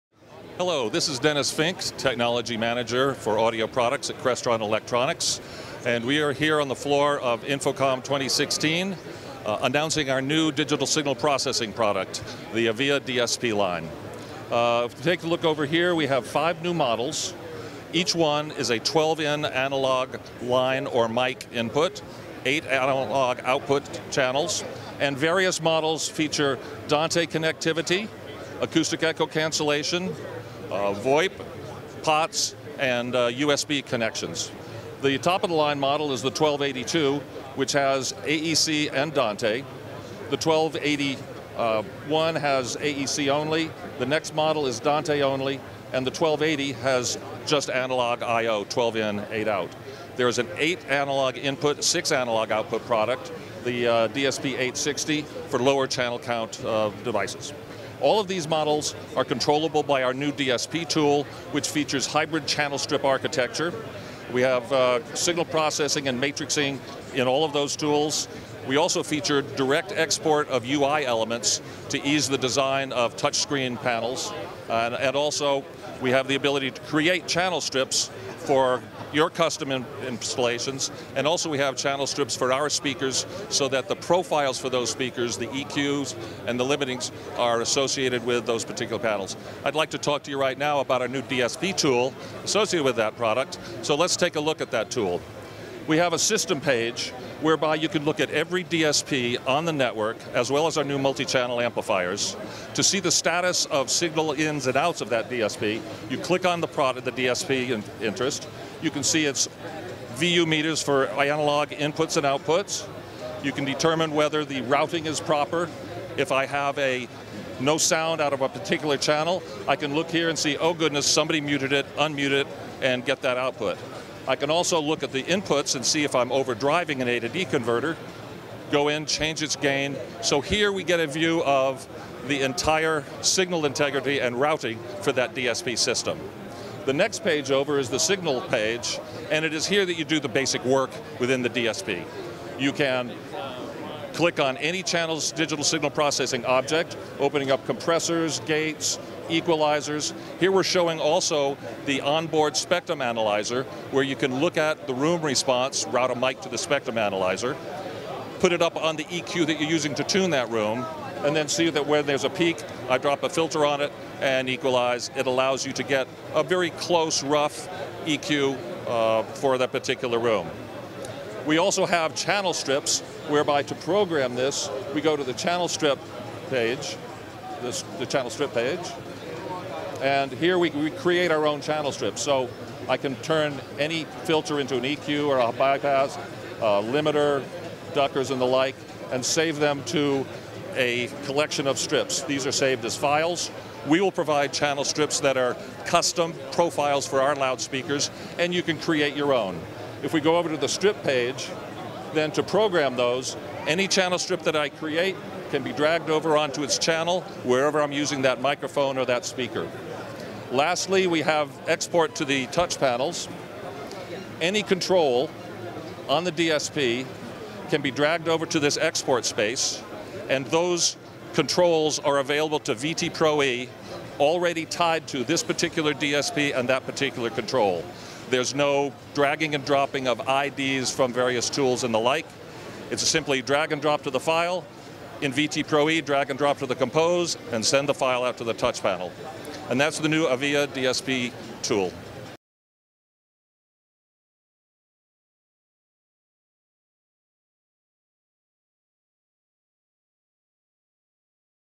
New Crestron DSP Tool
Dennis Fink, Technology Manager, Audio Products, demonstrates the exclusive new DSP Tool, which provides a massive leap forward in simplifying the process of programming, tuning, integrating, and expanding audio performance in meeting spaces.

Certain trademarks, registered trademarks, and trade names may be used in this video to refer to either the entities claiming the marks and names or their products. Crestron disclaims any proprietary interest in the marks and names of others.